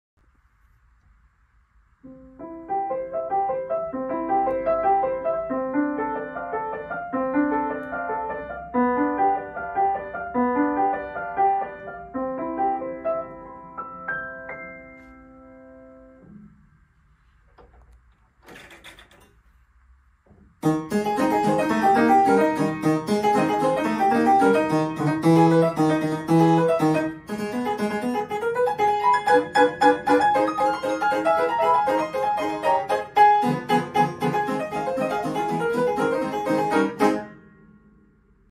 Honky-Tonk Piano with Mandolin Rail
This beautiful piano made by Yamaha is a full 46" tall, known as their "Style 31" model. This particular piano has the rare "mandolin attachment", which can make the piano sound like a honky-tonk with only the switch of a lever!

Includes a free tuning redeemable within the first 6 months of purchase, a 3 year in-store warranty (expandable to 5 years if a humidity control system is purchased along with the piano) and free delivery in Prince George, BC